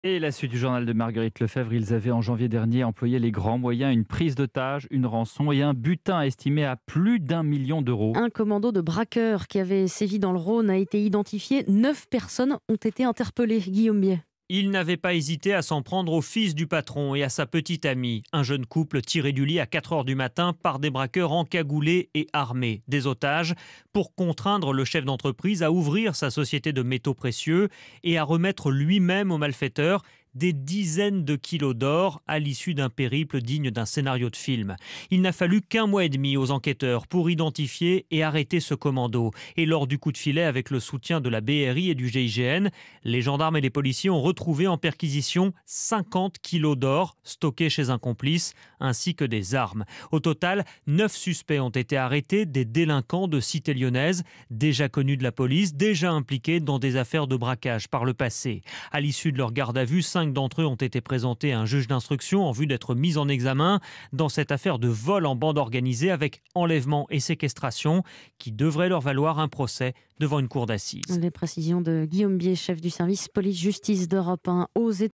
Prise d'otages, butin en or : 9 personnes soupçonnées d'un braquage près de Lyon arrêtées
Lors d'une prise d'otages en janvier dernier dans le Rhône, un commando de braqueurs avait mis la main sur plusieurs 20 à 30 kilos d'or. Le parquet de Lyon a annoncé lundi que 9 personnes avaient été interpellées.